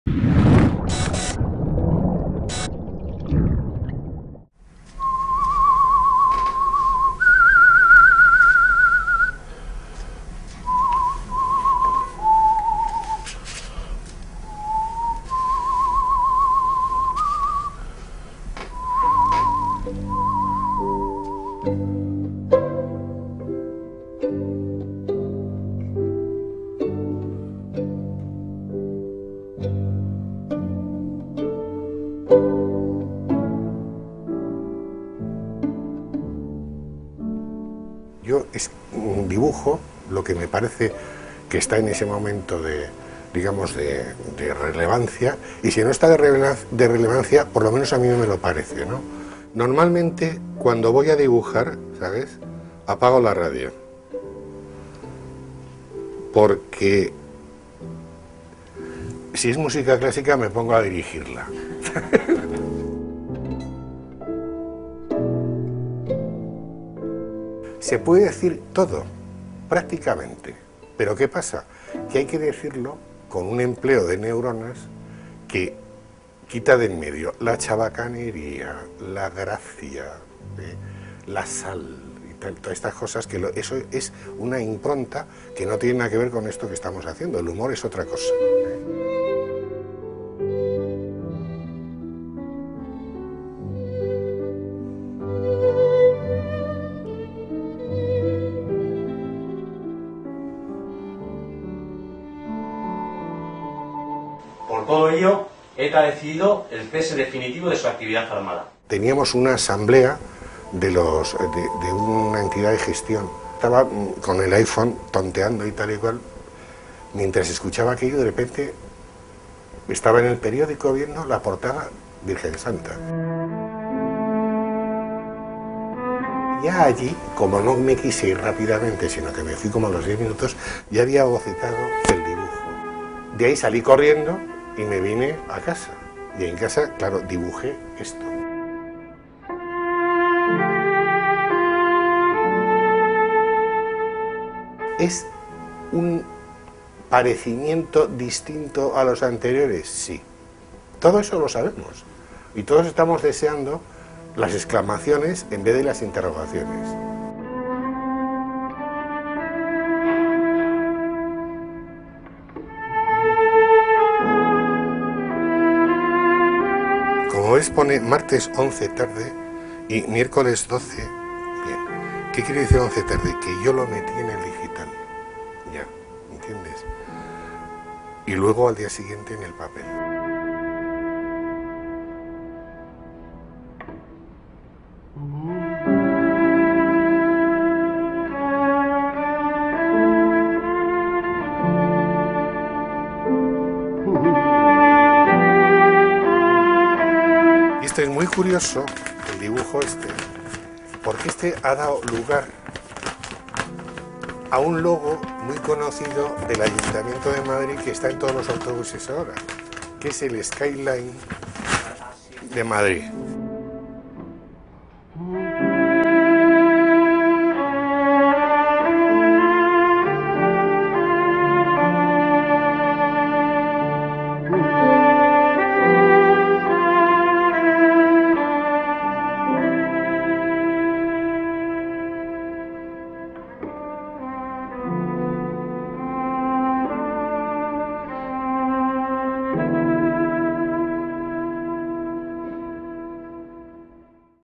¿Cómo se hace una viñeta de Forges?
El dibujante Antonio Fraguas, Forges, abre su oficina a EL PAÍS y rescata de su archivo momentos inolvidables.